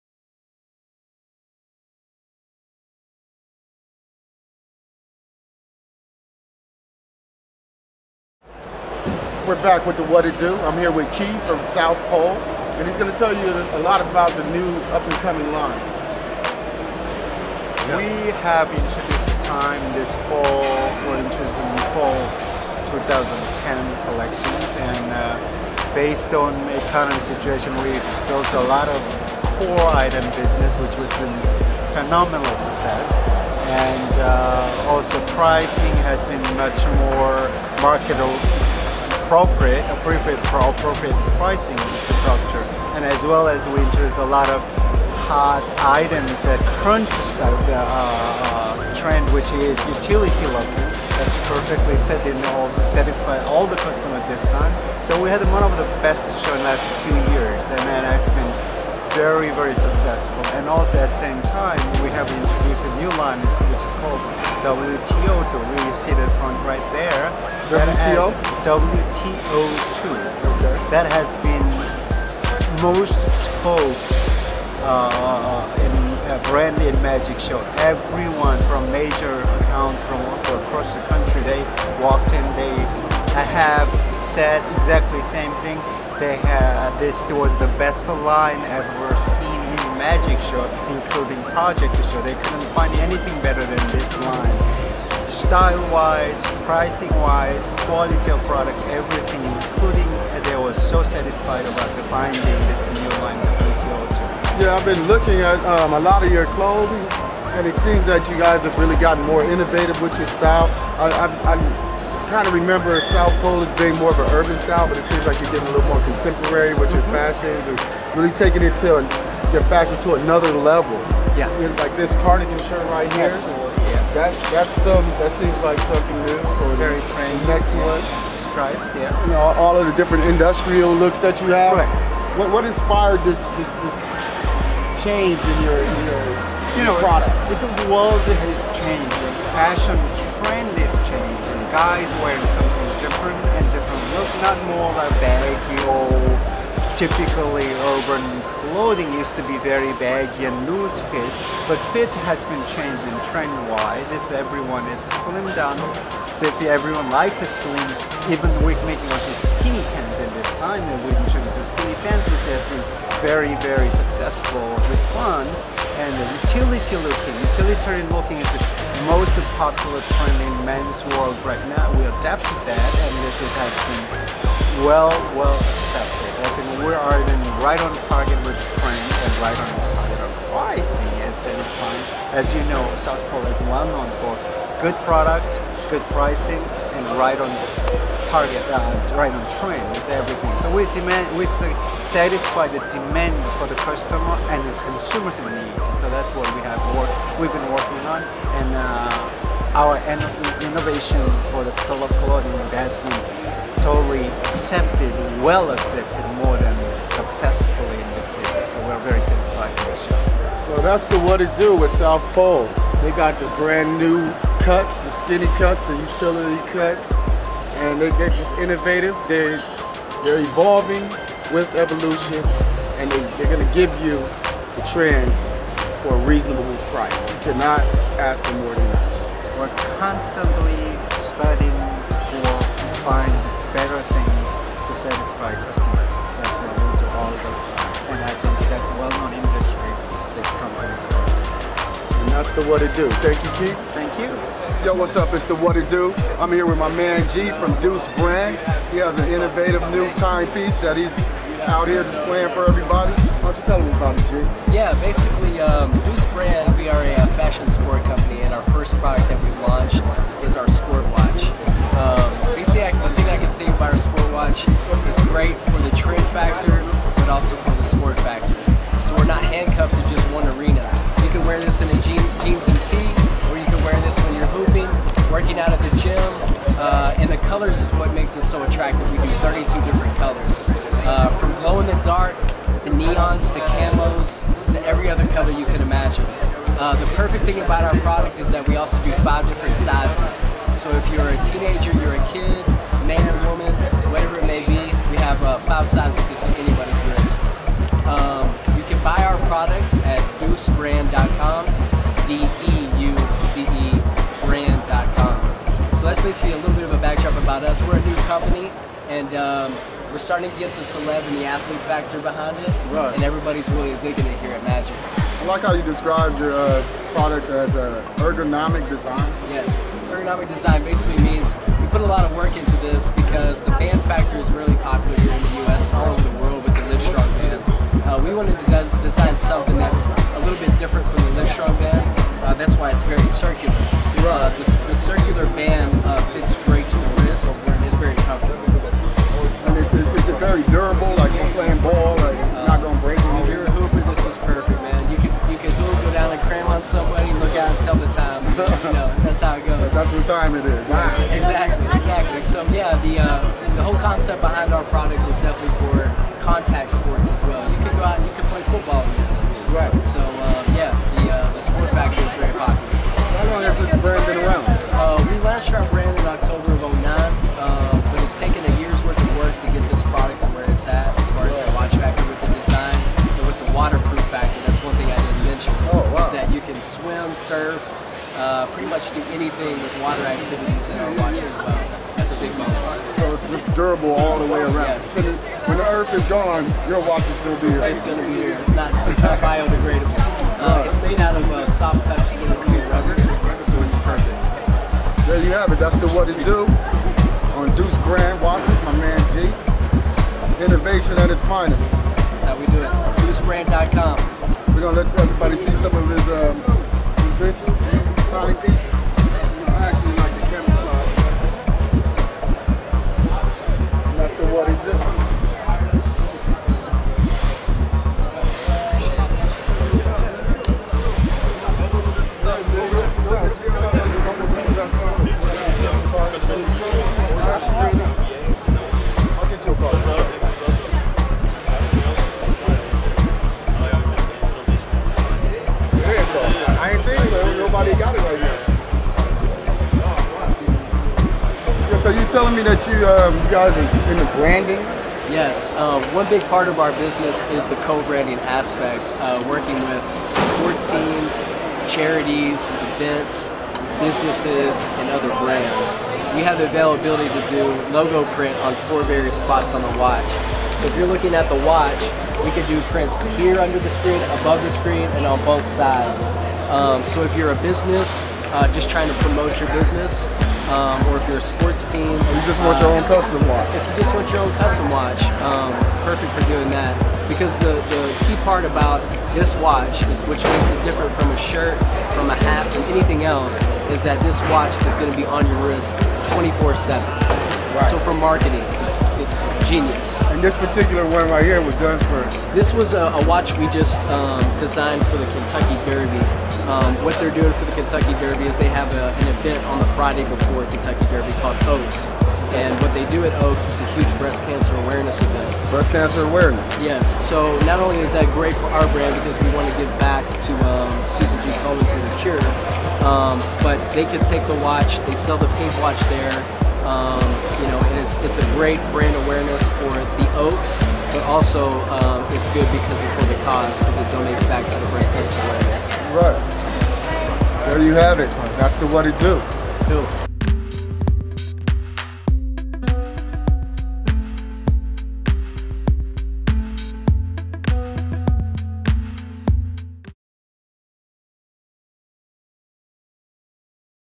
Magic Show Part Four
The What It Do's Host Mack Moore III interviews the SouthPole clothing line as well as a new Innovative brand called Deuce Brand.